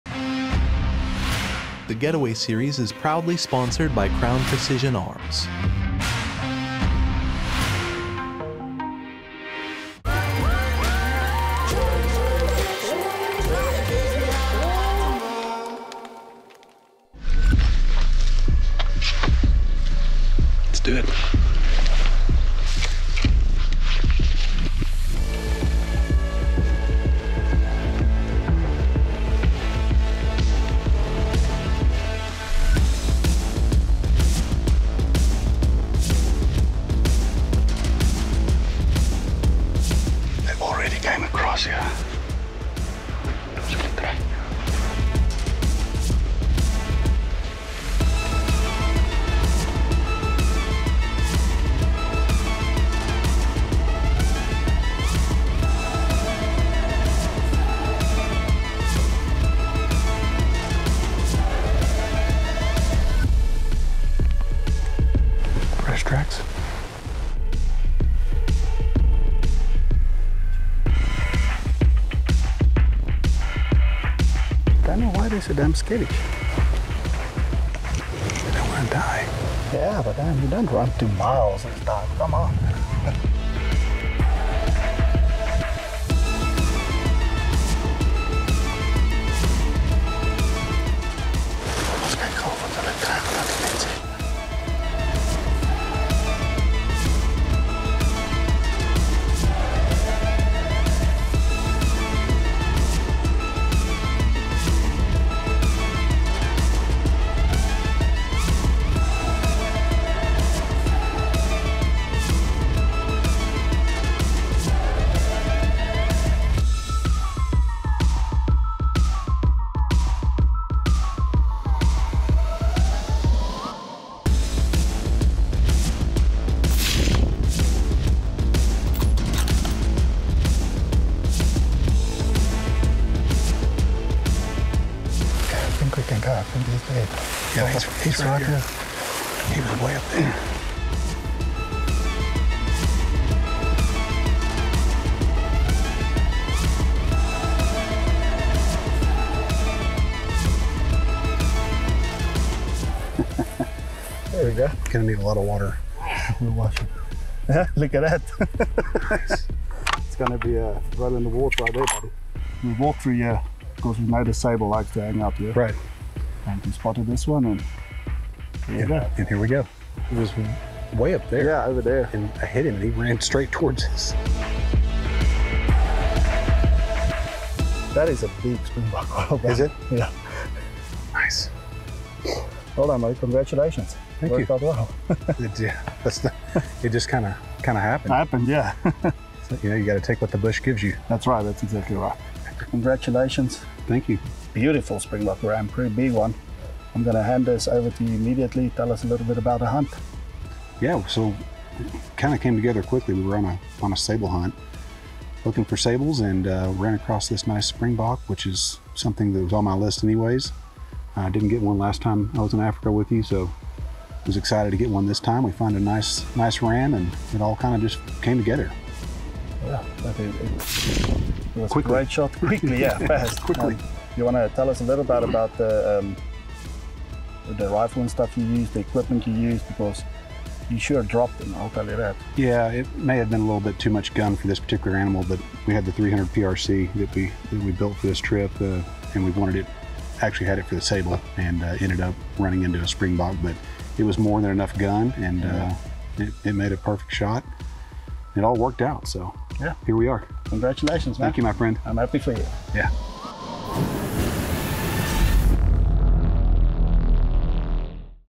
Epic Springbuck Hunt with the 300 PRC: A Precision Shooter's Dream
In this thrilling video, join us on an unforgettable journey to the heart of the African wilderness for an epic Springbuck hunt with the awe-inspiring 300 PRC rifle. If you're a precision shooter, this adventure is a dream come true!

Watch as we test the limits of accuracy and power, taking on the challenge of bagging these swift and elusive Springbuck with the utmost precision. Our skilled marksman, along with the remarkable capabilities of the 300 PRC, make for an exciting and intense hunt.

You'll witness breathtaking landscapes, up-close encounters with incredible wildlife, and the pulse-pounding moments that only a Springbuck hunt can provide. Plus, we'll share some valuable insights into the 300 PRC rifle, discussing its features, ballistics, and the gear that contributes to making it a precision shooter's dream.